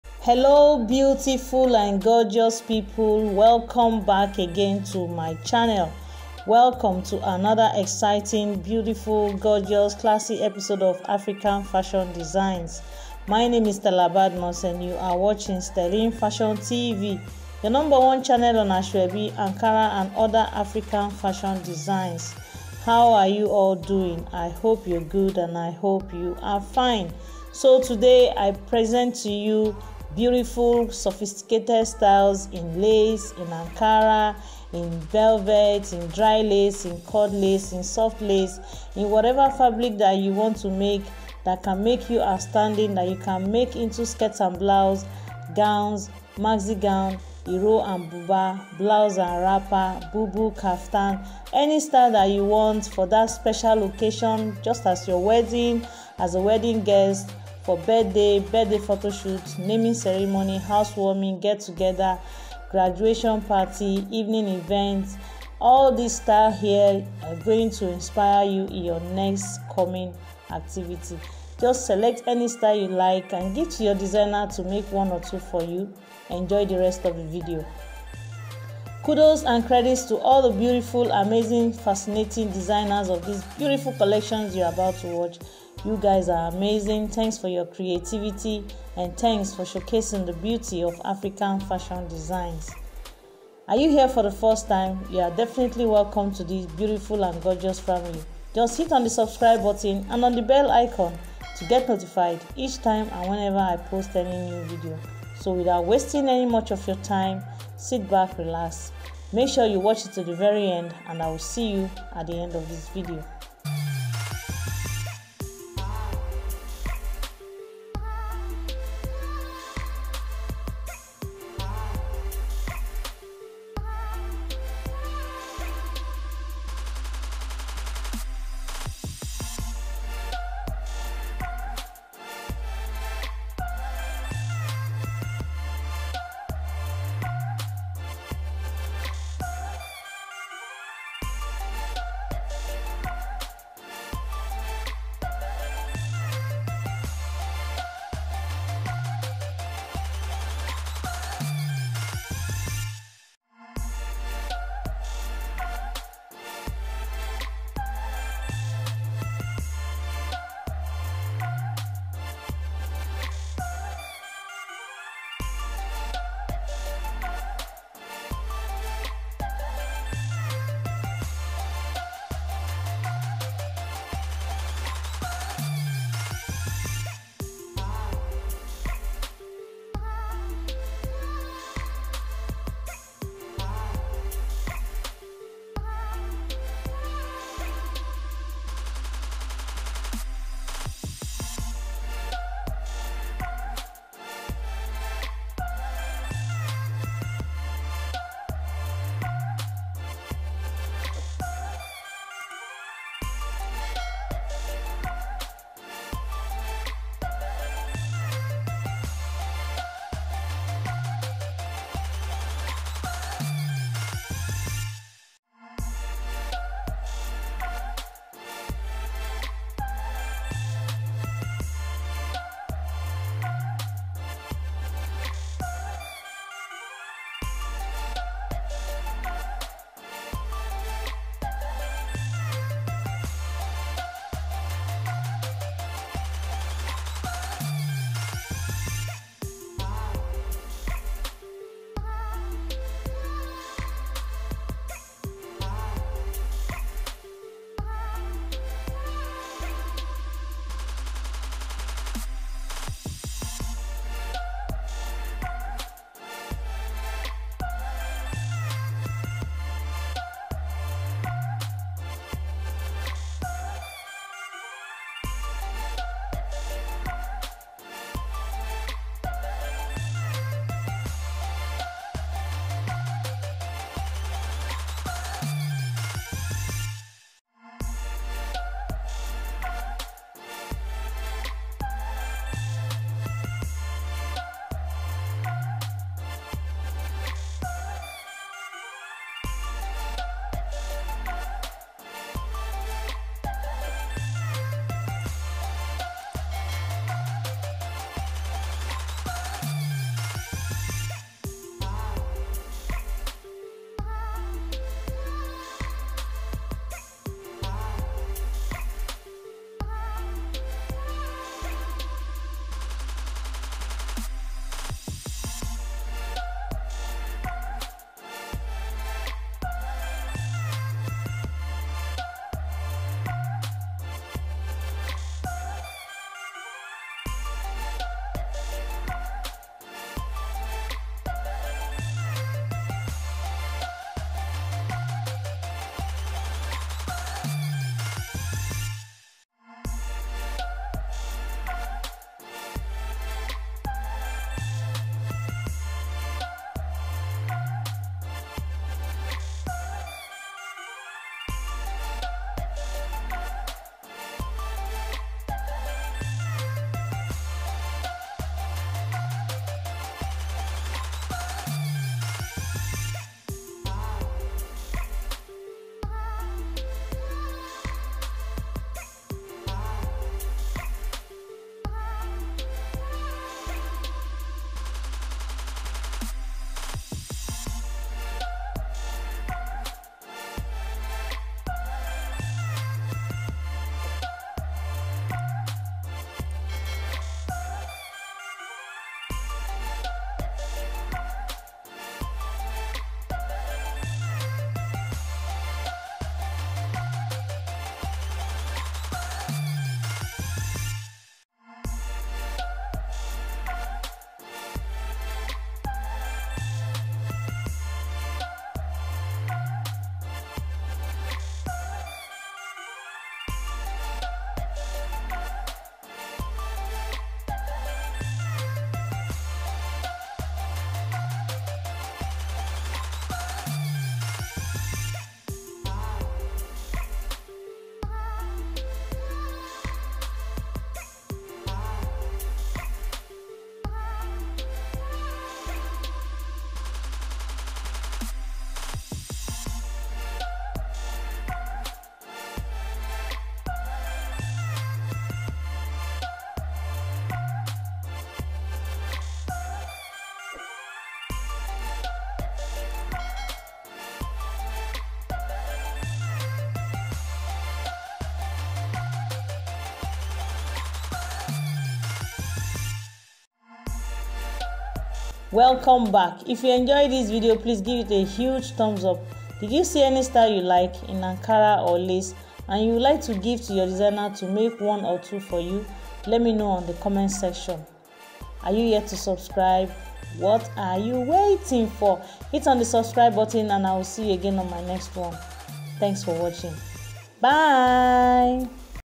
African Clothing Styles for Ladies || Ankara Styles || Asoebi Styles for Lace || Boubou Gown Styles
Hello Fashionistas, today you will be watching Ankara Styles, Lace Styles Iro and Buba, Blouse and Wrapper, Boubou and Kaftan Styles you won't want to miss, so sit back and enjoy watching to the very end and let me know on the comment section what you think.